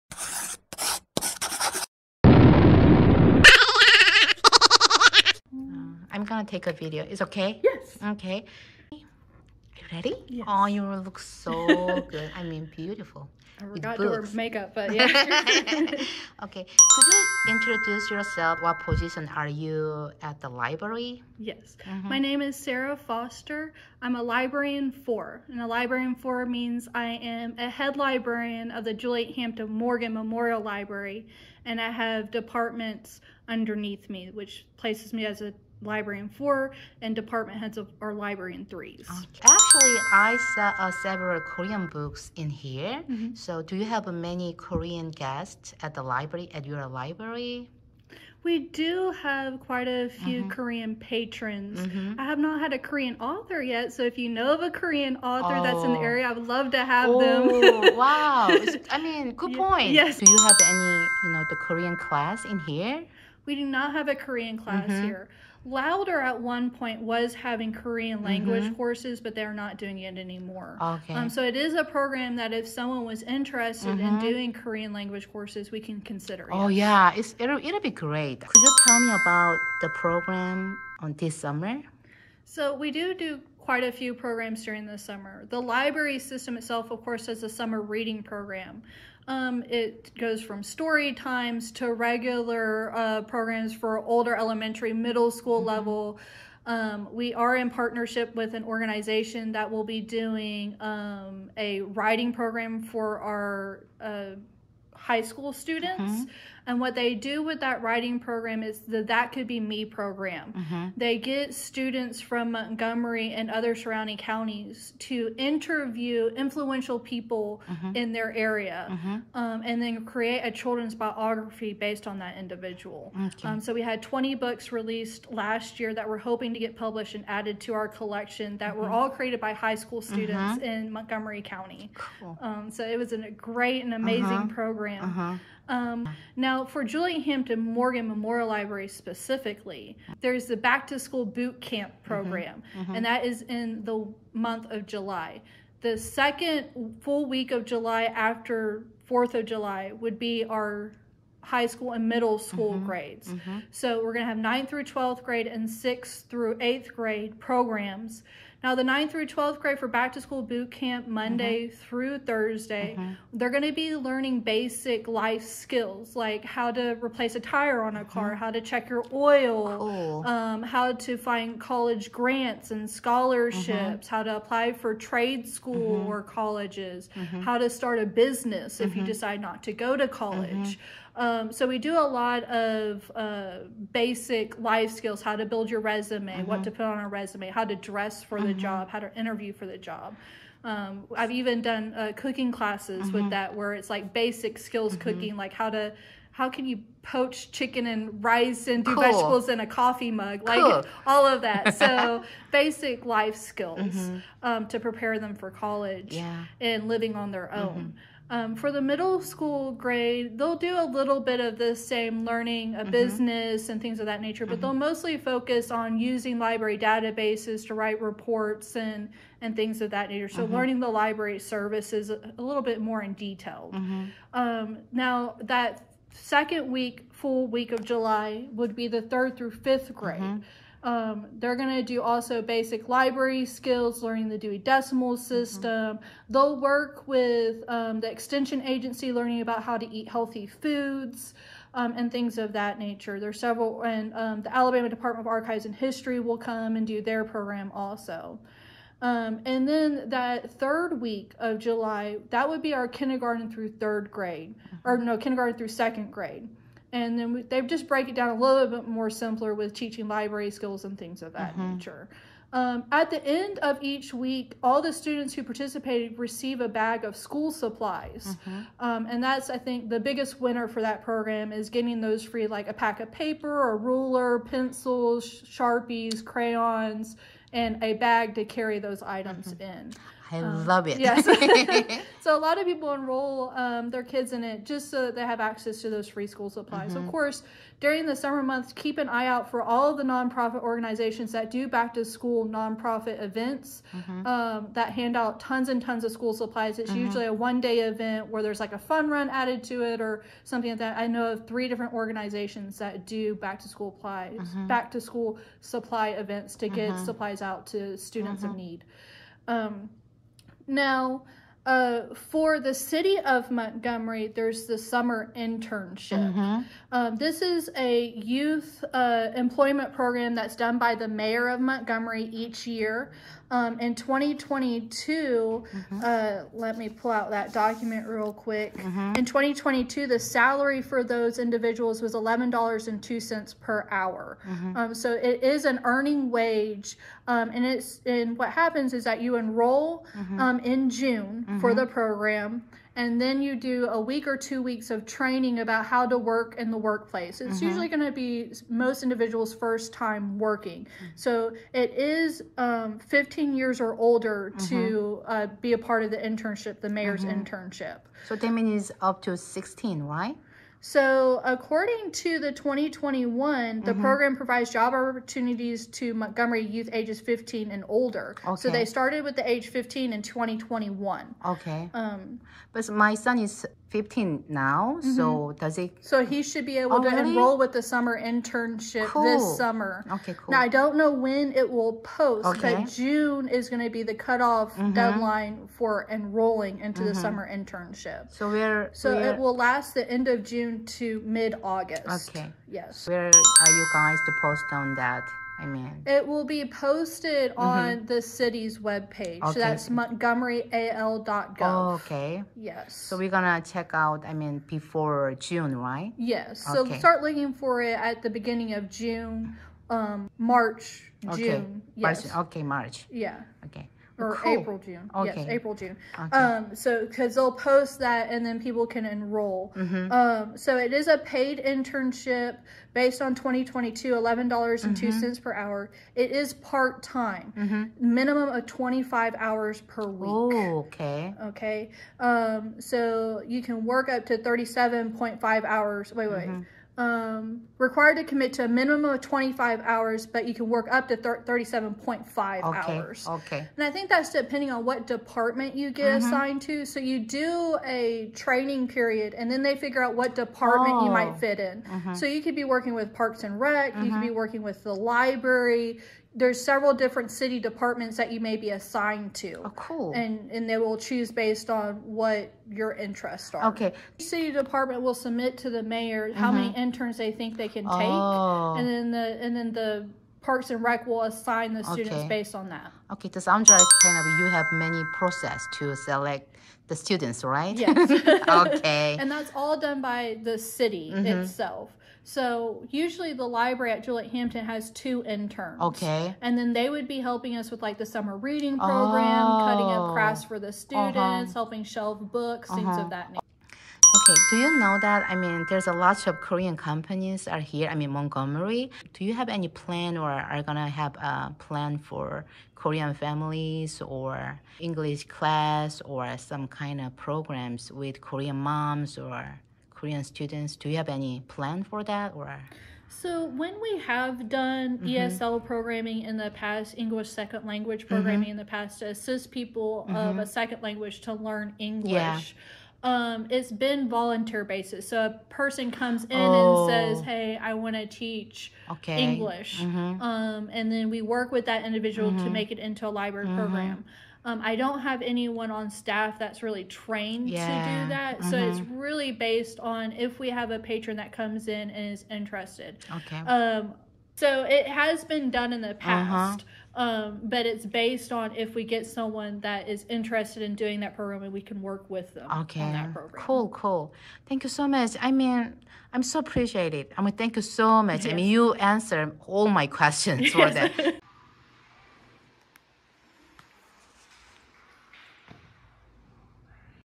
[미국 일상 브이로그] 미국에서 살아남기, 미국주부 브이로그, 미국도서관, 미국도서관사서, 미국여름방학, 미국 썸머캠프, 미국 썸머 프로그램, 미국이민생활, 미국생활현실,미국인터뷰
[최강 오지랖퍼 안젤라가 간다간다간다. ] 도서관 사서와의 한판 승부~~!!
Juliette Hampton Morgan Memorial Library 미국도서관 수석사서와의 여름방학 프로그램에 대한 인터뷰~~!! CallmeAngela 체널에선 보기힘든 진지한 토크쇼.